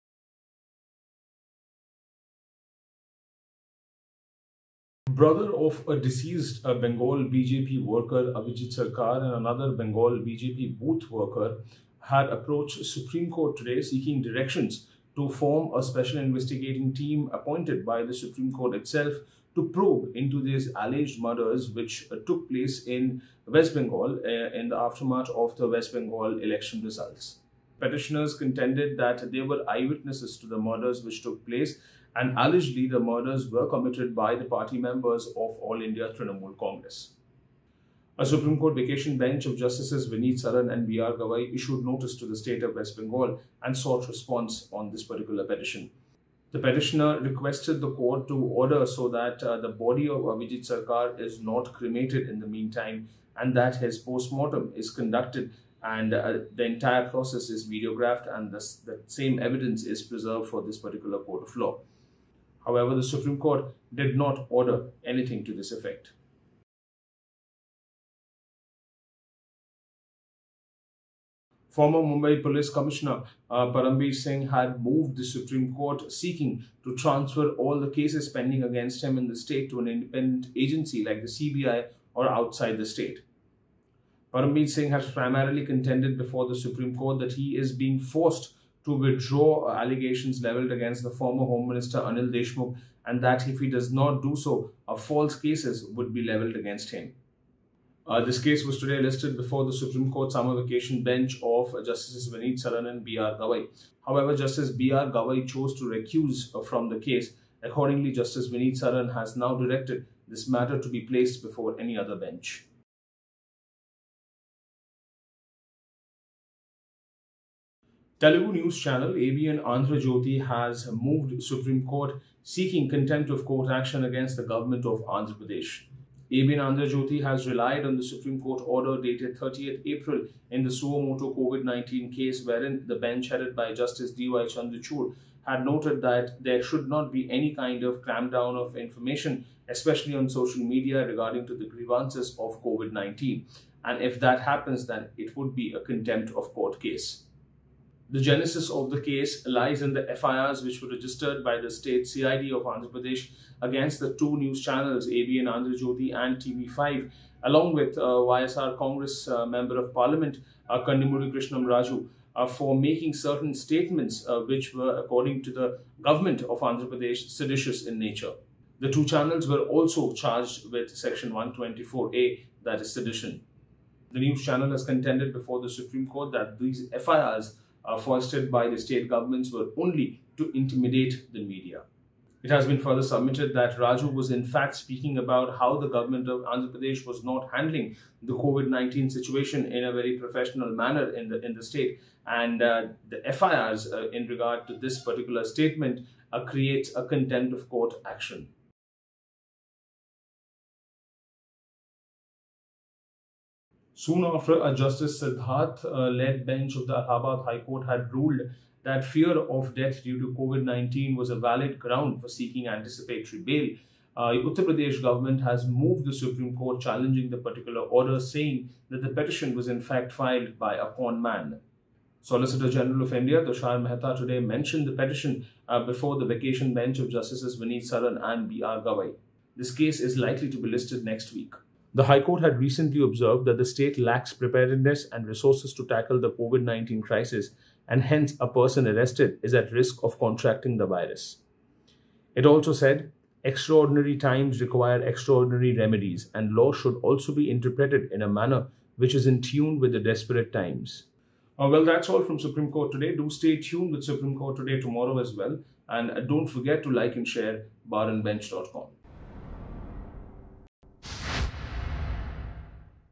"Murder" of BJP Bengal workers, Param Bir Singh's plea, Contempt plea against Andhra govt: SC Today
Supreme Court Today: Daily Update from SC (18 MAY 2021)

Bar & Bench brings you a daily update video from the Supreme Court on the key cases heard, observations made and judgments delivered.

Supreme Court seeks response from West Bengal govt on plea for CBI probe into killing of two BJP workers

Justice BR Gavai recuses from hearing plea by Param Bir Singh against Maharashtra govt inquiries

Andhra Govt "intimidating media": Telugu news channel files contempt of court petition for violating Supreme Court order on COVID-19 coverage

"Filed by a conman:" UP govt moves Supreme Court against Allahabad HC order holding fear of death due to COVID-19 ground for anticipatory bail